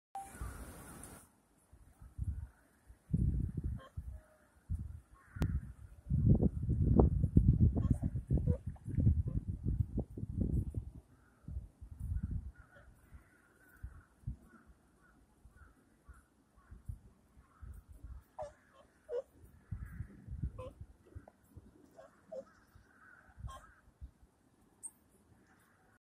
20220101 与兵衛沼 白鳥のあくび
画面右下の白鳥があくびしています
餌を食べて眠くなっちゃったみたい…